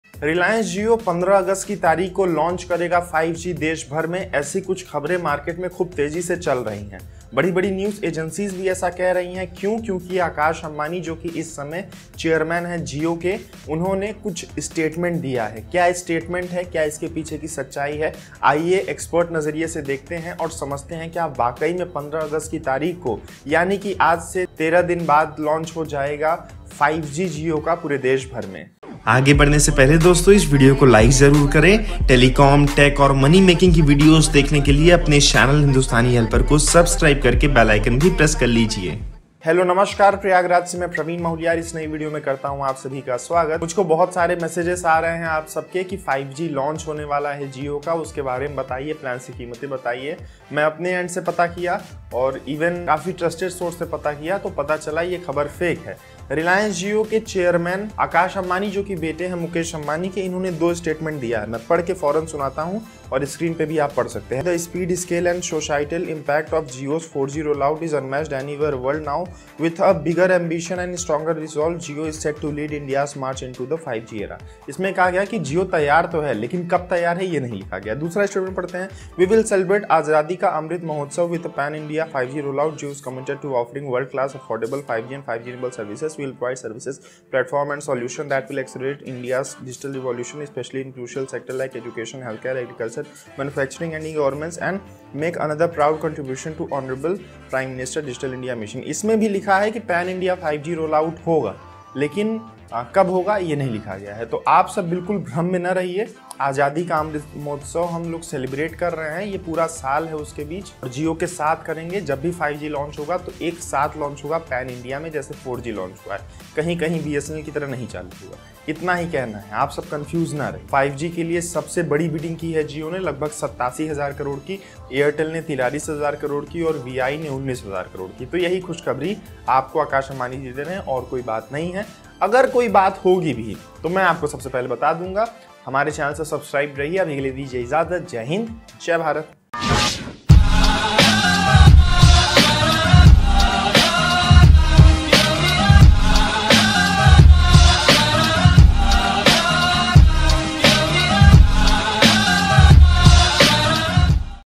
खुद AKASH AMBANI ने किया ऐलान 🔥 Reality of JIO'S 5G Launch on 15 August 2022
Reliance Jio, the number one telecom operator in the country, could launch 5G very soon. The telco has mostly stayed quiet about its 5G plans and trials, unlike its competitors, Vodafone Idea (Vi) and Bharti Airtel. Akash Ambani, the newly appointed Chairman of Reliance Jio, said, “We will celebrate ‘Azadi ka Amrit Mahotsav’ with a pan India 5G rollout. Jio is committed to offering world-class, affordable 5G and 5G-enabled services.”

Jio has purchased enough spectrum in this spectrum auction to rollout 5G services widely. The telco has spent over Rs 88,078 crore in this spectrum auction to get the kind of airwaves that none of the other telcos has. Jio is the sole operator, which has 700 MHz airwaves right now. It is definitely going to give Jio an edge over the competition.